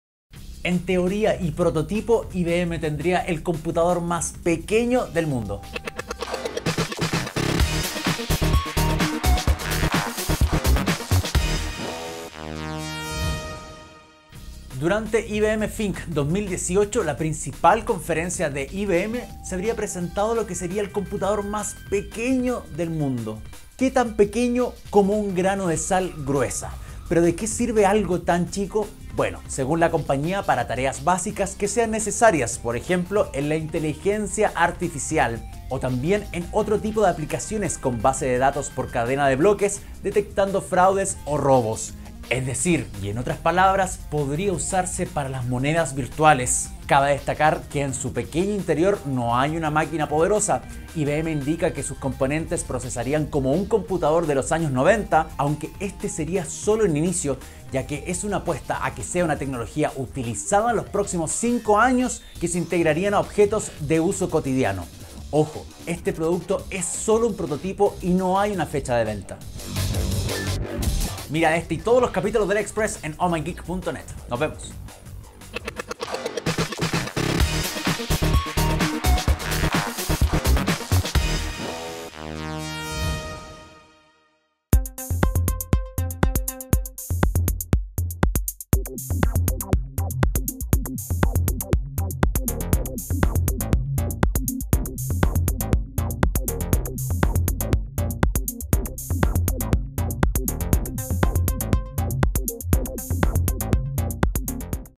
Este es el prototipo del COMPUTADOR más PEQUEÑO del MUNDO 🖥
¡Del tamaño de un grano de sal gruesa! Así sería el prototipo del computador más pequeño del mundo que IBM habría presentado durante Think 2018, la principal conferencia de esta compañía. Los detalles te los contamos en este OhMyGeek! Express.